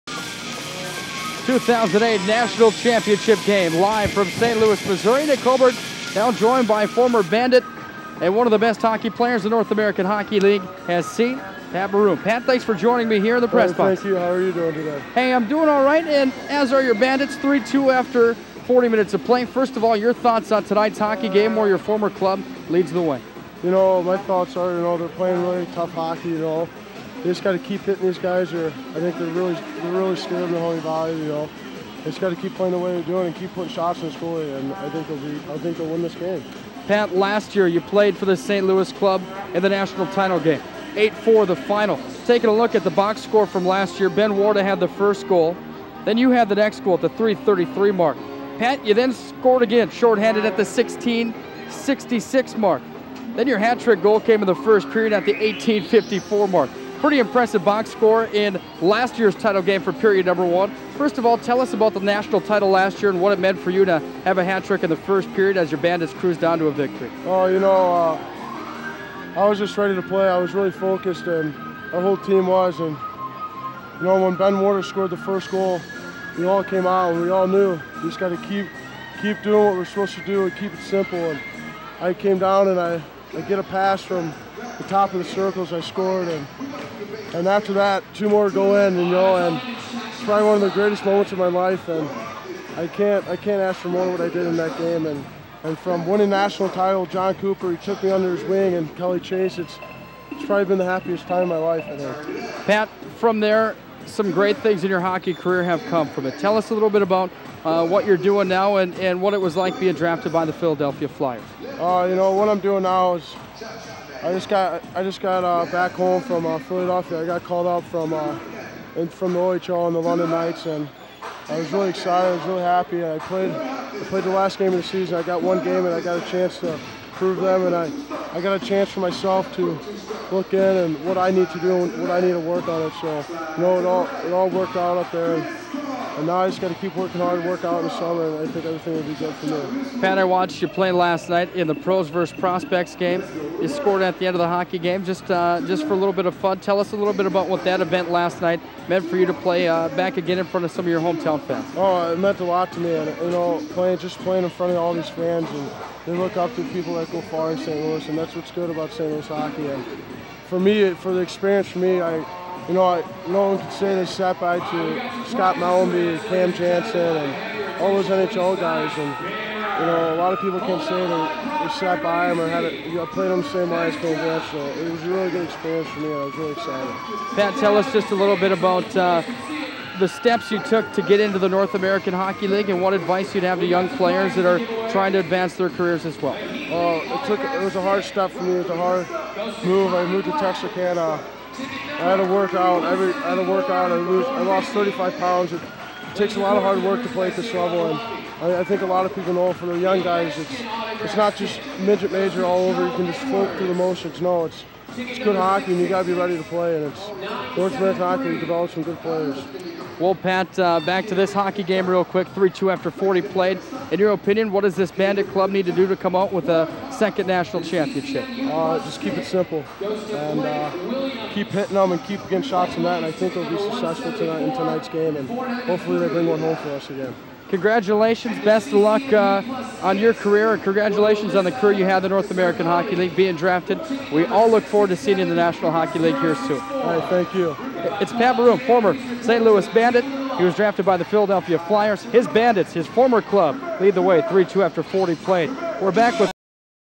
Nick Hulberg Resume-- Intermission
Nick Hulberg Resume-- Intermission Interview with Pat Maroon.